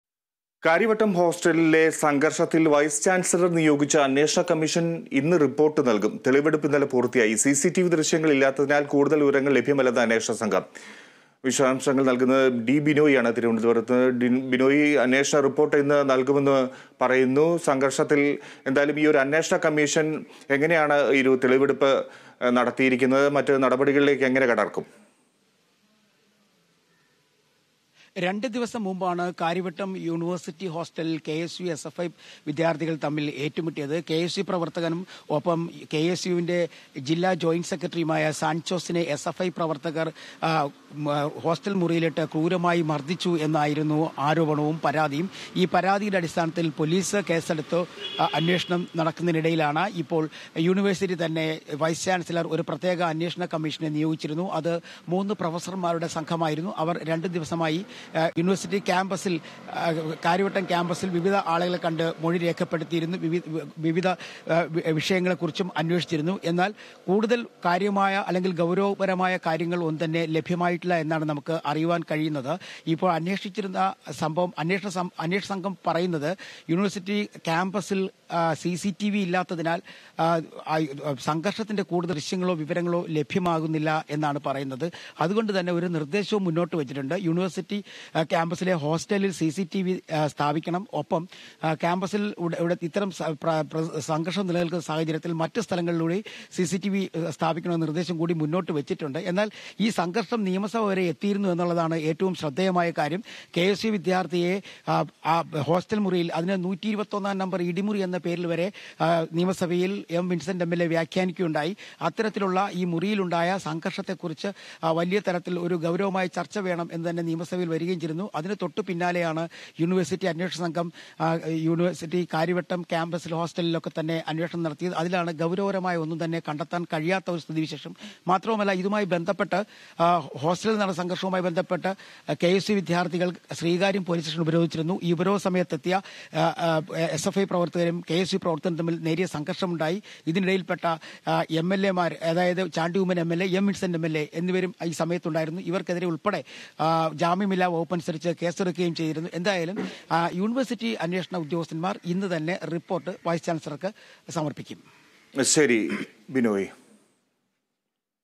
ഹോസ്റ്റല്‍ സംഘര്‍ഷം;CCTV ദൃശ്യങ്ങള്‍ ഇല്ലാത്തതിനാല്‍ കൂടുതല്‍ വിവരങ്ങള്‍ ലഭ്യമല്ലെന്ന് അന്വേഷണസംഘം
കാര്യവട്ടം ഹോസ്റ്റലിലെ സംഘര്‍ഷത്തില്‍ വൈസ് ചാന്‍സിലര്‍ നിയോഗിച്ച അന്വേഷണ കമ്മീഷന്‍ ഇന്ന് റിപ്പോര്‍ട്ട് നല്‍കും.തെളിവെടുപ്പ് ഇന്നലെ പൂര്‍ത്തിയായി. സിസിടിവി ദൃശ്യങ്ങള്‍ ഇല്ലാത്തതിനാല്‍ കൂടുതല്‍ വിവരങ്ങള്‍ ലഭ്യമല്ലെന്ന് അന്വേഷണസംഘം

About COA